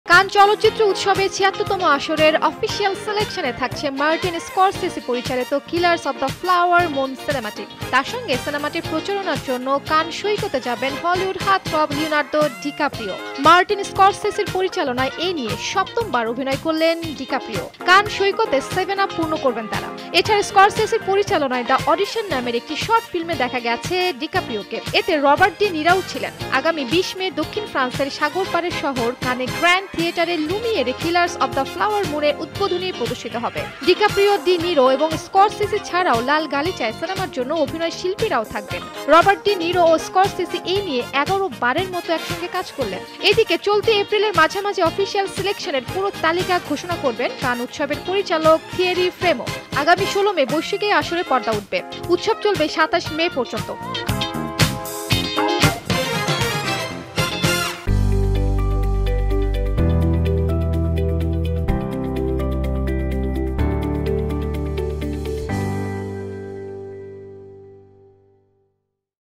কান উৎসবে 'কিলারস অব দ্য ফ্লাওয়ার মুন' | Cannes Film Festival | Showbiz Tonight
কান চলচ্চিত্র উৎসবের ৭৬তম আসরের অফিসিয়াল সিলেকশনে থাকছে মার্টিন স্করসেসি পরিচালিত ‘কিলারস অব দ্য ফ্লাওয়ার মুন’ সিনেমাটি। তার সঙ্গে সিনেমাটির প্রচারণার জন্য কানসৈকতে যাবেন হলিউড হার্টথ্রব লিওনার্দো ডিক্যাপ্রিও। মার্টিন স্করসেসির পরিচালনায় এ নিয়ে সপ্তমবার অভিনয় করলেন ডিক্যাপ্রিও। কানসৈকতে সেভেন-আপ পূর্ণ করবেন তারা! এছাড়া স্করসেসির পরিচালনায় ‘দ্য অডিশন’ নামের একটি শর্টফিল্মে দেখা গেছে ডিক্যাপ্রিওকে। এতে রবার্ট ডি নিরোও ছিলেন। আগামী ২০ মে দক্ষিণ ফ্রান্সের সাগরপাড়ের শহর কানে গ্র্যান্ড থিয়েটার লুমিয়েরে ‘কিলারস অব দ্য ফ্লাওয়ার মুন’-এর উদ্বোধনী প্রদর্শনী হবে। ডিক্যাপ্রিও, ডি নিরো এবং স্করসেসি ছাড়াও লালগালিচায় সিনেমার অন্য অভিনয়শিল্পীরাও থাকবেন। রবার্ট ডি নিরো ও স্করসেসি এ নিয়ে ১১ বারের মতো একসঙ্গে কাজ করলেন। এদিকে চলতি এপ্রিলের মাঝামাঝি অফিসিয়াল সিলেকশনের পুরো তালিকা ঘোষণা করবেন কান উৎসবের পরিচালক থিয়েরি ফ্রেমো। আগামী ১৬ মে বৈশ্বিক এই আসরে পর্দা উঠবে। উৎসব চলবে ২৭ মে পর্যন্ত।